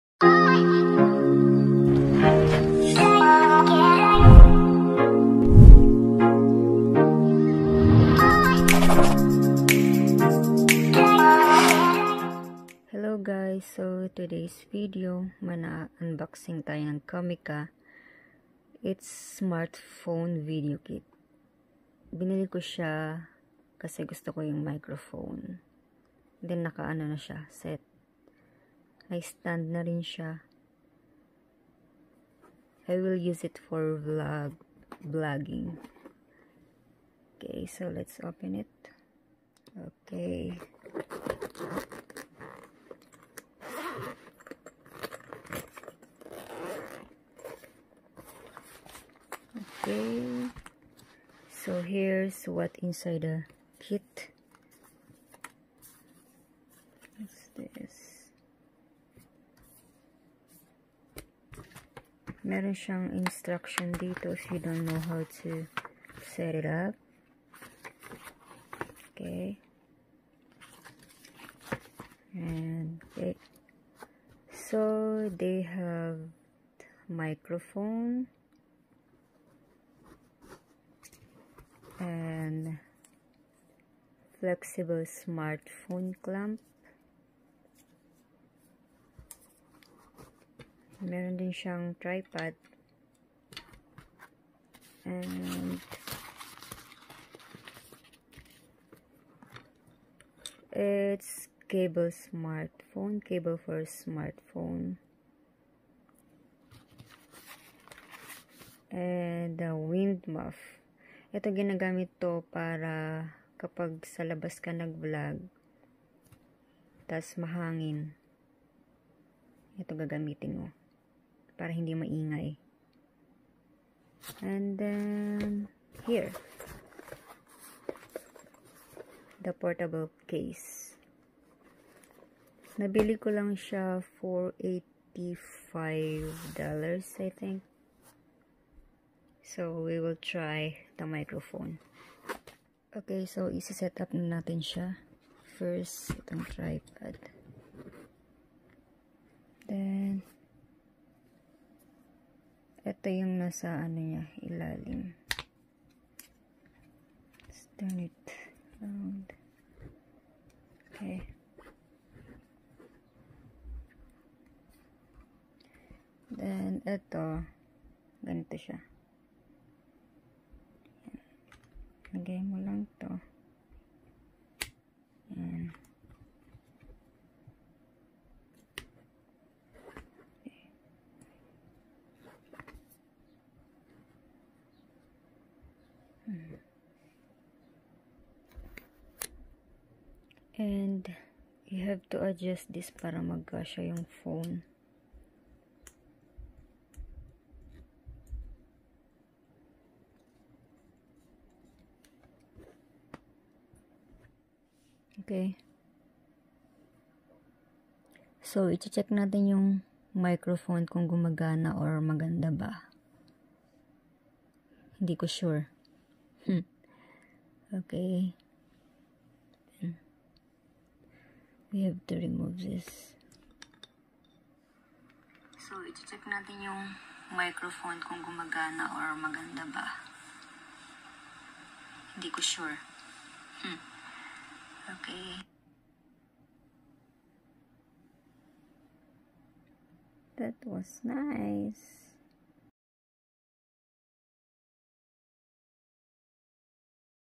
Comica Smartphone Video Kit|CVM-10-K2
I bought this from Amazon! It cost $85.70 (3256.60 in peso)

I was impressed with overall quality of the comica CVM-VM10-K2,this is good for blogging,for mukbang,asmr😉 and easy to set up..

phone I used to Video- samsung s20 ultra (back camera)
where I edit my Videos -Kinemaster